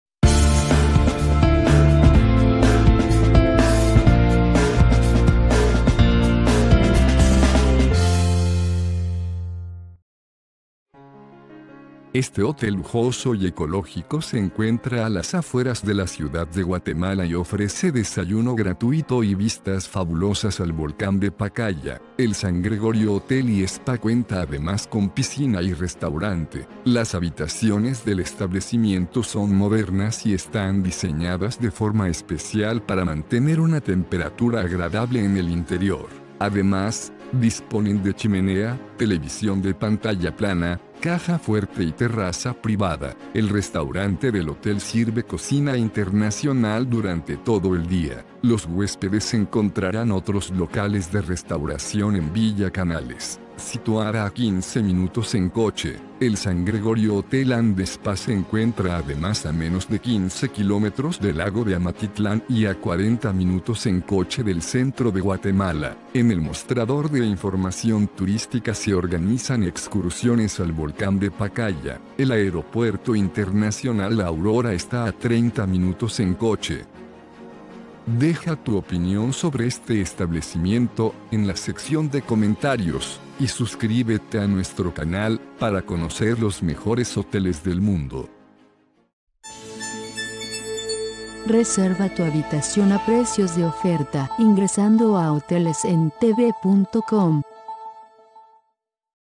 San Gregorio Hotel & SPA en Villa Canales, Guatemala
San Gregorio Hotel & SPA es un adorable Hotel de Villa Canales con servicio de habitaciones, salas de reuniones / banquetes y bar. Este alojamiento que ingresó al sistema en el año [año de ingreso] ofrece 10 habitaciones con ducha, bañera y caja fuerte.

Ingresa en nuestro portal para obtener descuentos en San Gregorio Hotel & SPA.